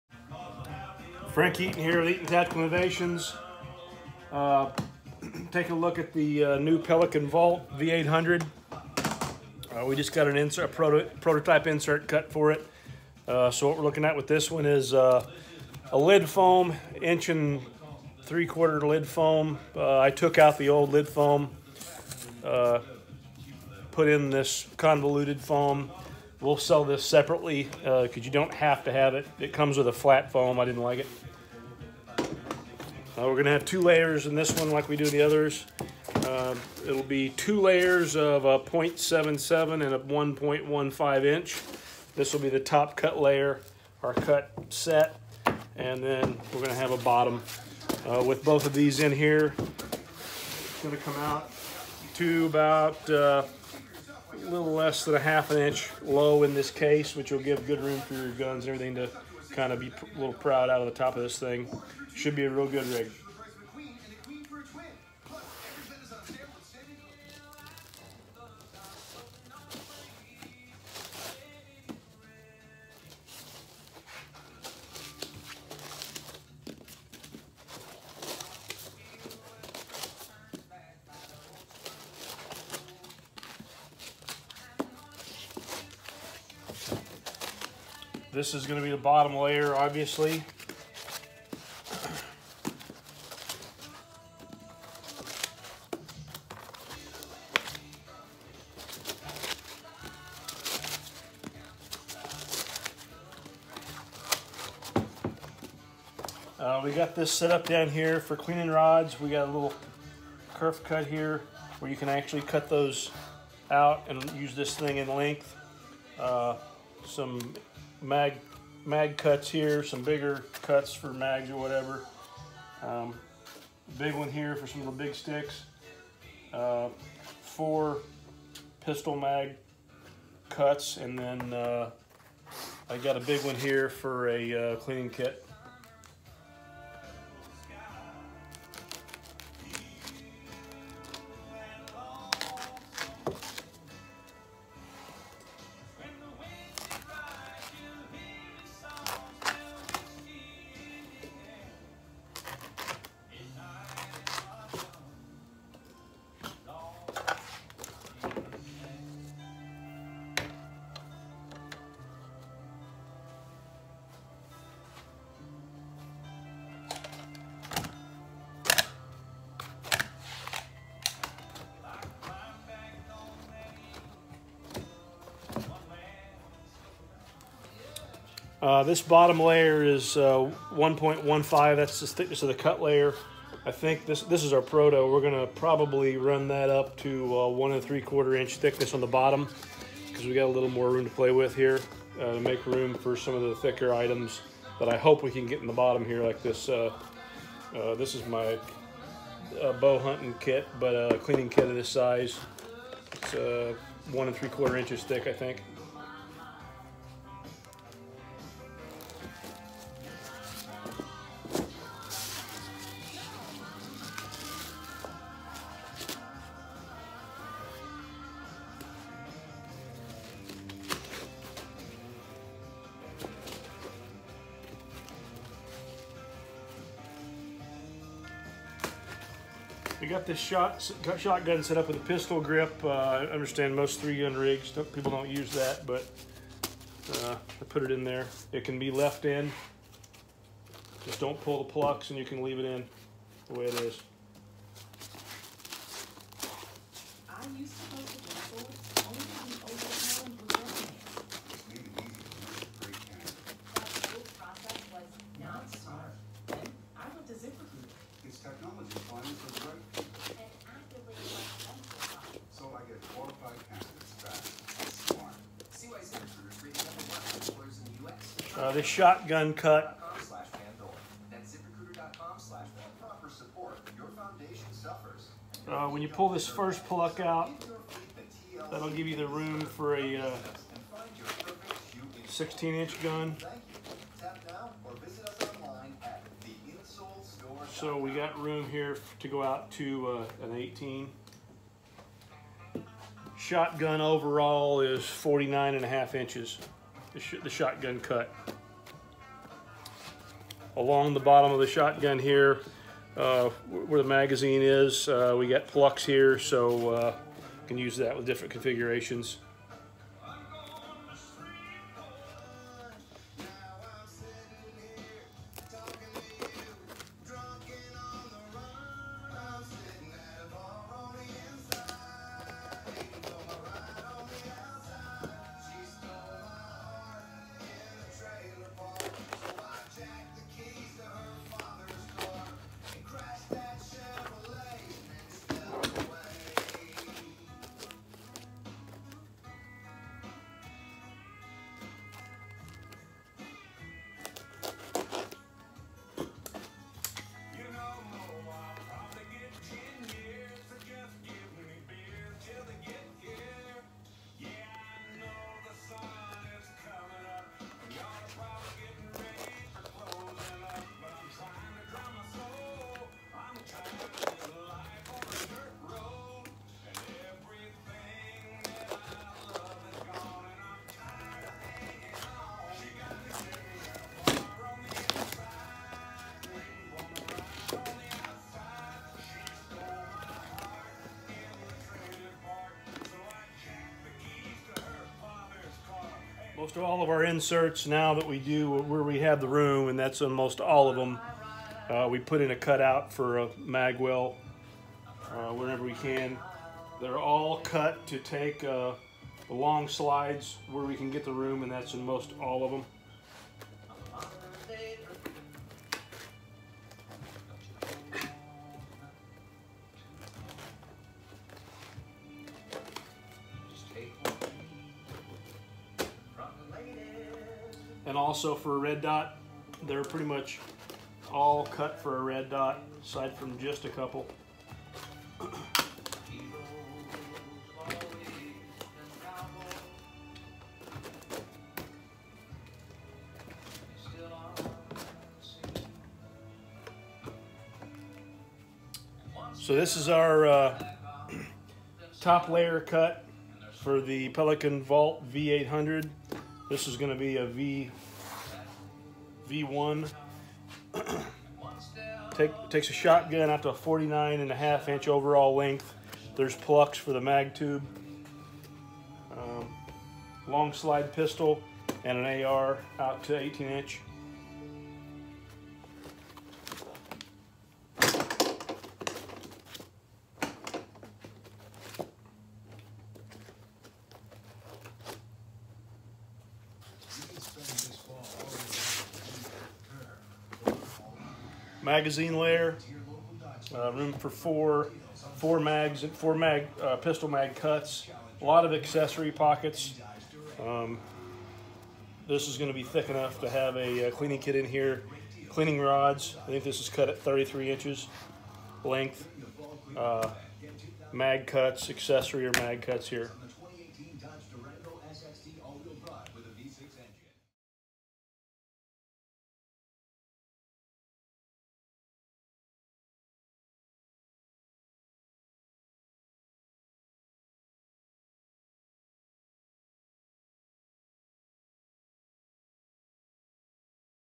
GUNFORMZ Case Set Up for Pelican Vault V800
Setting up this new insert for the Pelican Vault V800- video was filmed in segments. This doesnt include the glue up, thats a different video.